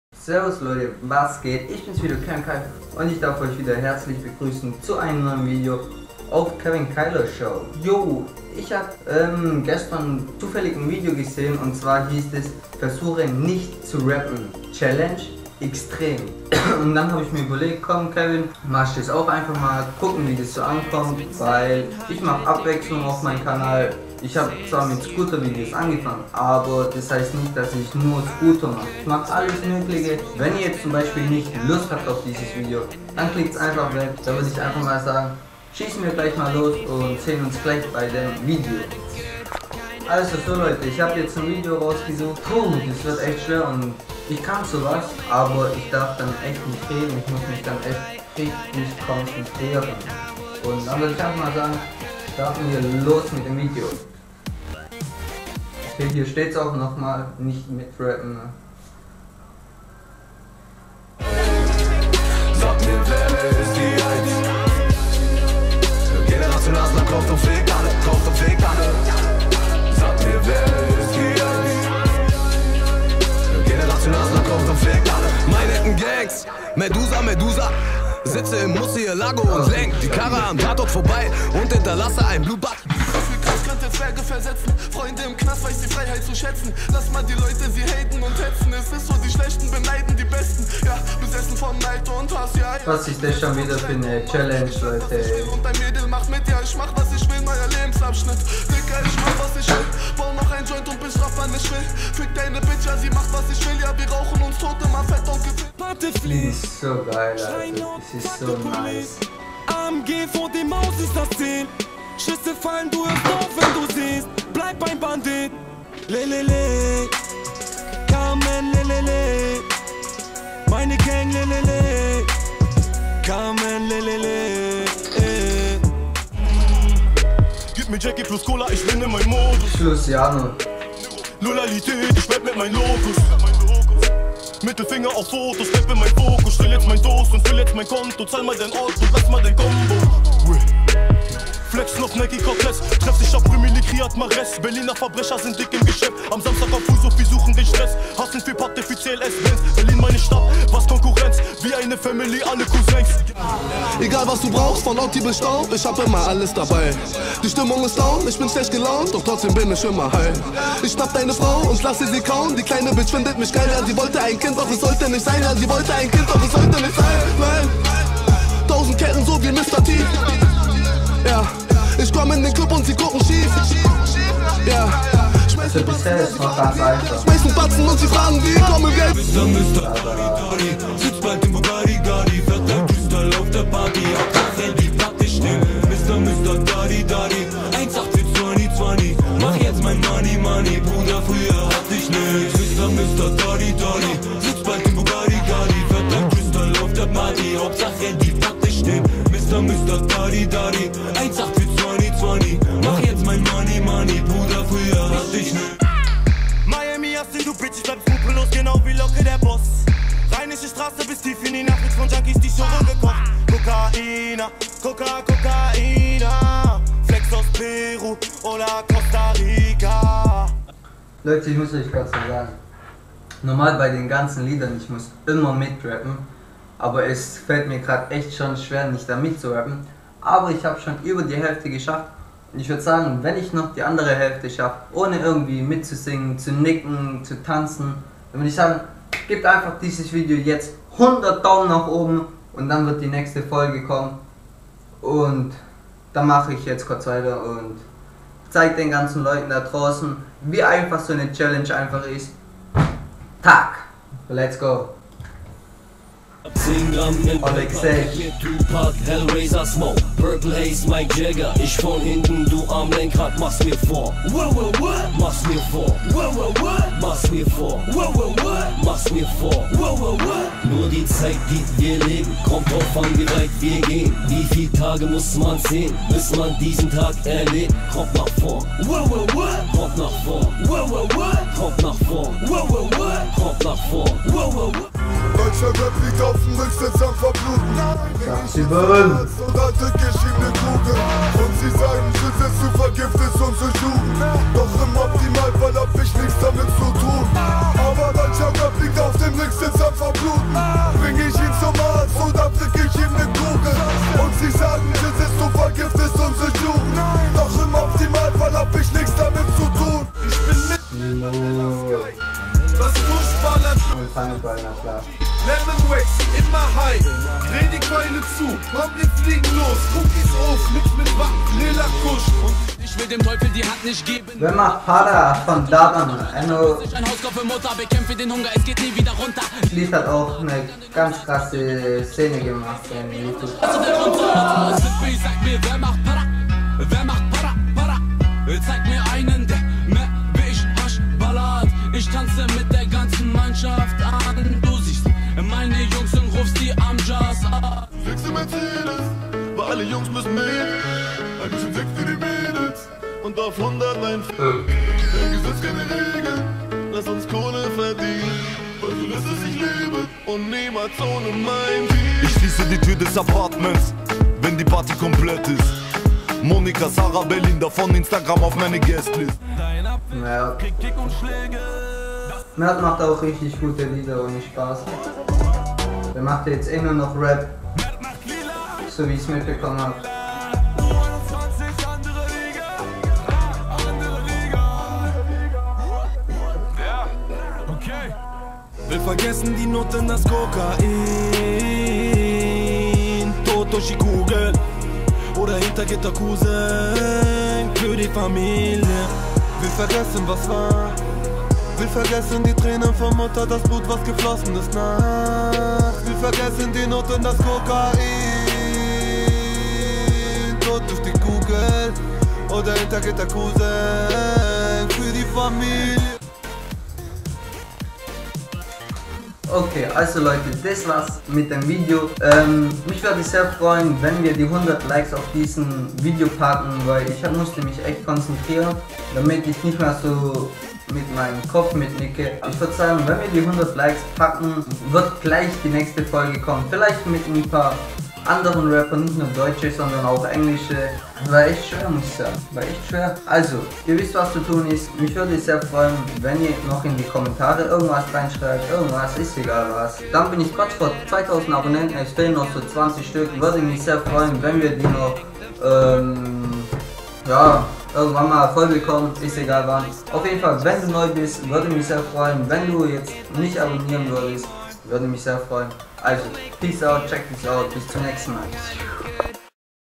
VERSUCHE NICHT ZU RAPPEN!!! GERMAN RAP🙄
Versuche nicht mit zu Rappen Extrem Schwer ihr schafft es zu 100% nicht! Zeigt eure Reaktion in die Kommentare und schreibt ob ihr es geschafft habt oder nicht! ;)

---ANAQUDA SHOP---

---INSTAGRAM SETUP / TRICKS SEITEN---

---FACEBOOK FANPAGE---

kevinkailer

Freue mich über allen Neuen Menschen die AKTIV auf meinen Kanal sind! ;)